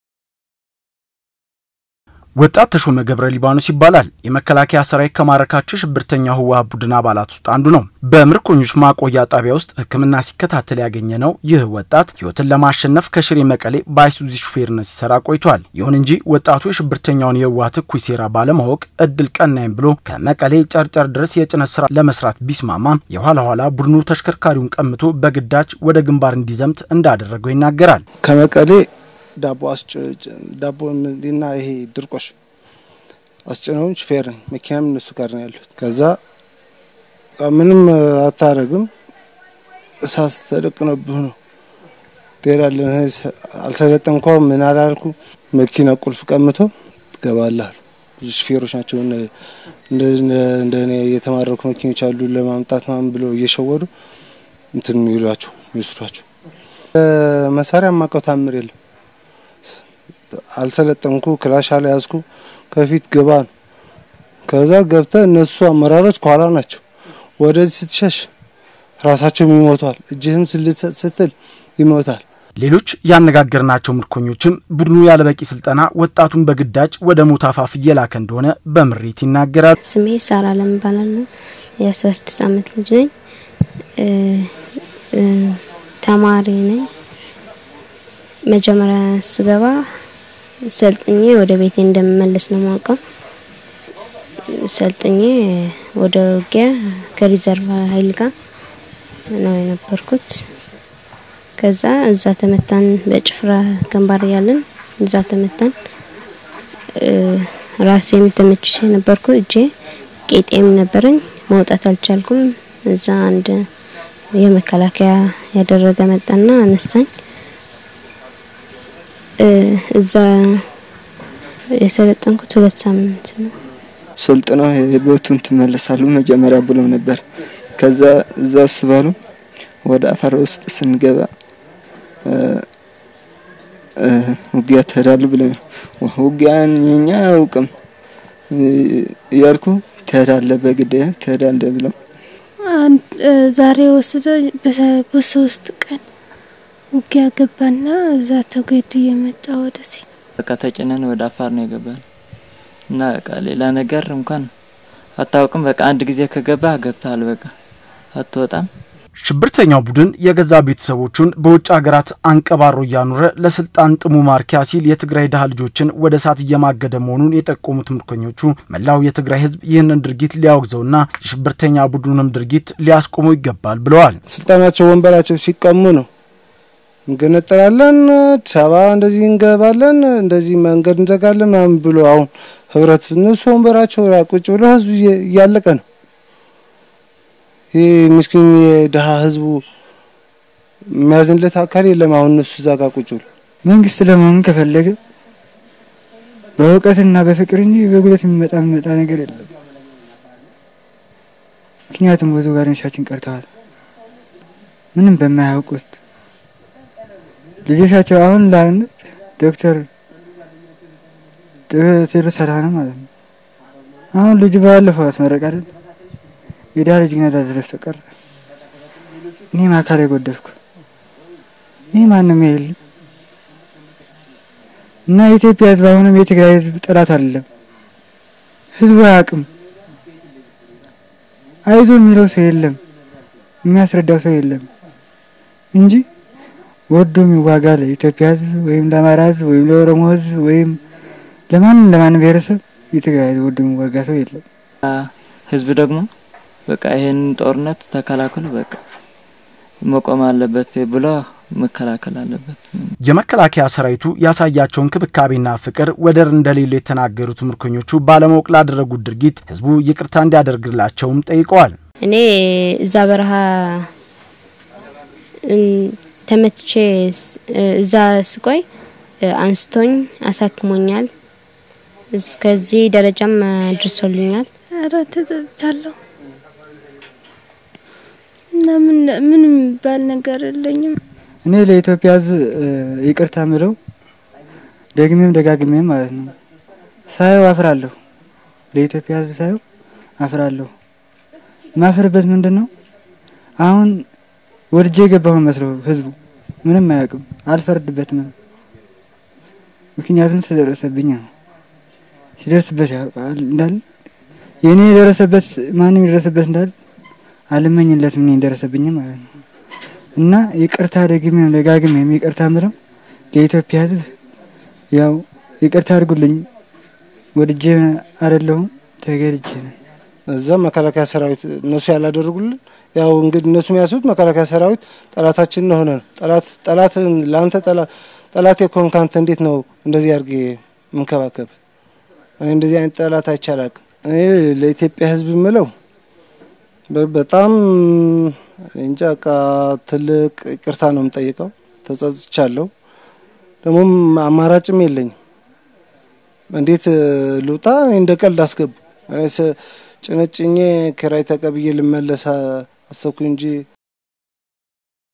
ምርኮኞች ሚካኤል አዲስ ሚዲያ ኔትወርክ ነሐሴ 13/2013 ዓ.ም
ሽብርተኛው የህወሃት ቡድን ታዳጊዎችን ጨምሮ ወጣቶችን በግዳጅ ወደ ጦር ግንባር እየላከ መሆኑን ምርኮኞች ተናገሩ፡፡
ከመከላከያ ሰራዊቱ ተገቢውን እንክብካቤና ፍቅር እያገኙ እንደሆነ የተናገሩት ምርኮኞቹ ባለማወቅ ለፈፀሙት ጥፋትም ህዝቡን ይቅርታ  ጠይቀዋል፡፡